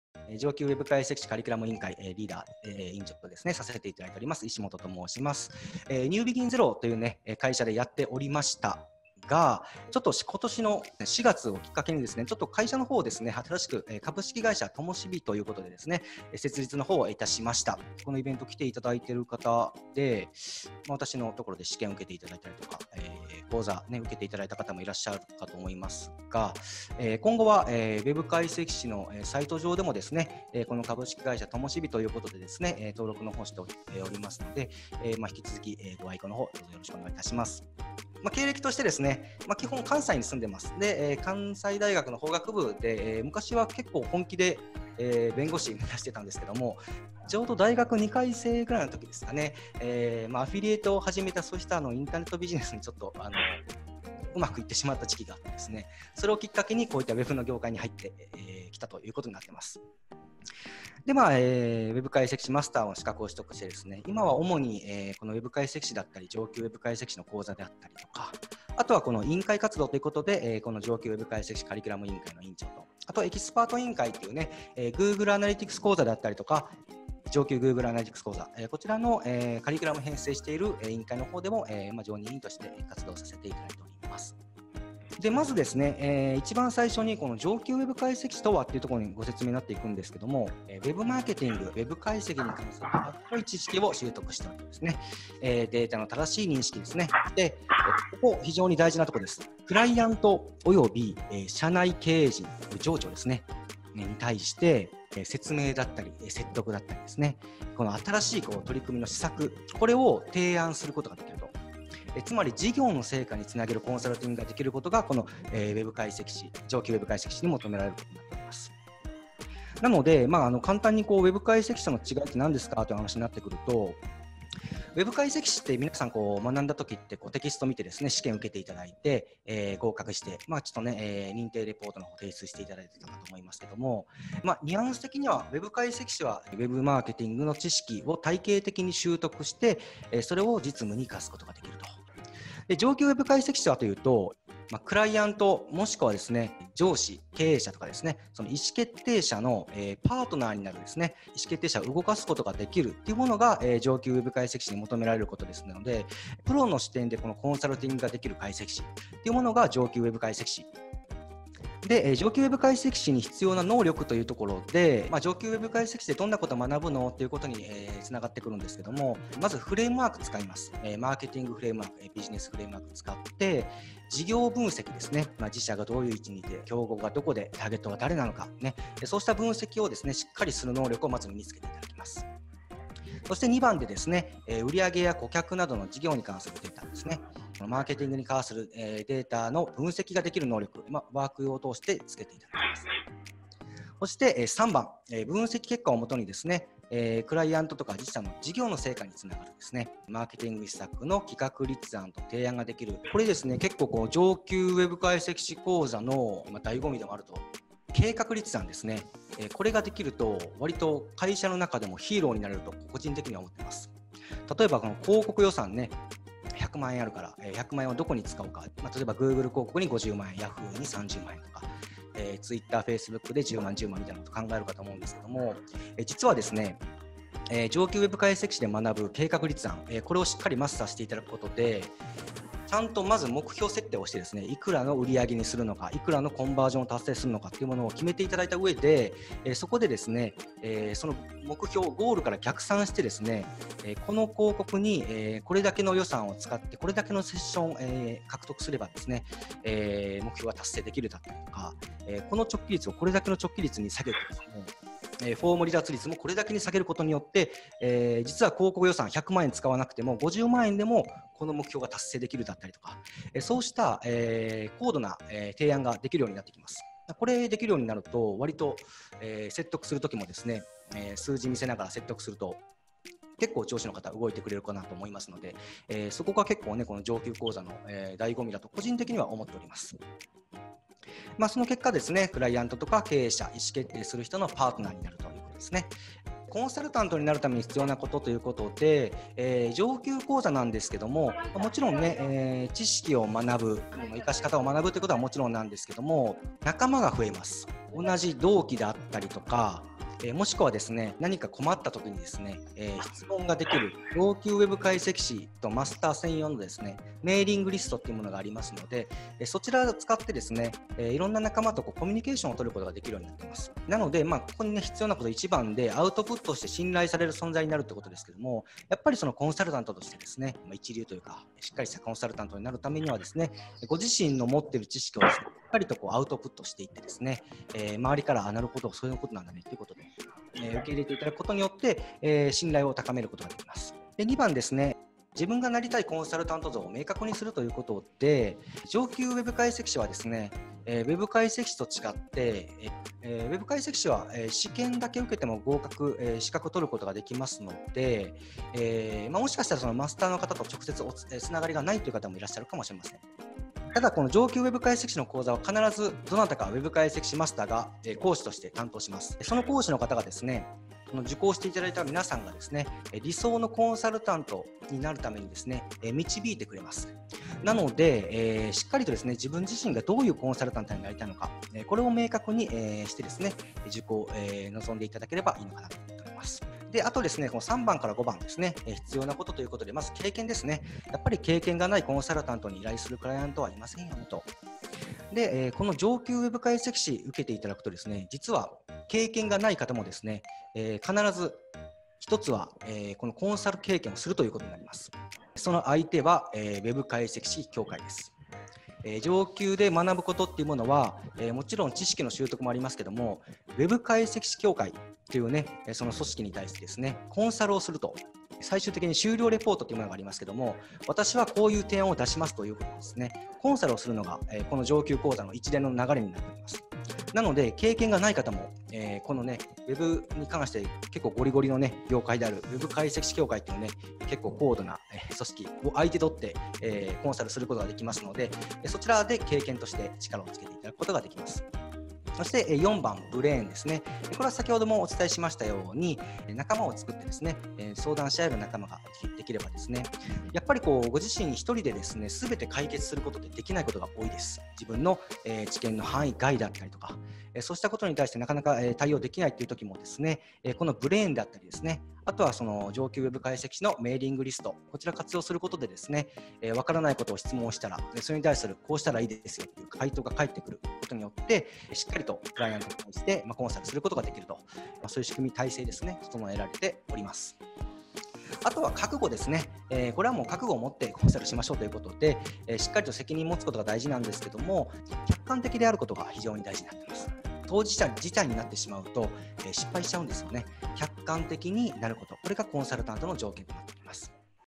【上級ウェブ解析士講座紹介ver.01】プロの視点でコンサルティングができるパートナーになろう
「ウェブサイトを変えるだけじゃ、正直、意味ないよね」
ビジネスを動かし、事業を成功させるための戦略コンサルタント。
上級ウェブ解析士とはどうあるべきか、何ができるか、この動画を見れば、今まで「上級」の意味が分からなかった方も、上級ウェブ解析士を目指したくなるかと思います。

内容（8分）
・スピーカー自己紹介
・上級ウェブ解析士とは
・ウェブ解析士との違い
・上級ウェブ解析士講座で習得できること
・上級ウェブ解析士とコンサルタント

【上級ウェブ解析士の学習内容と取得の流れ】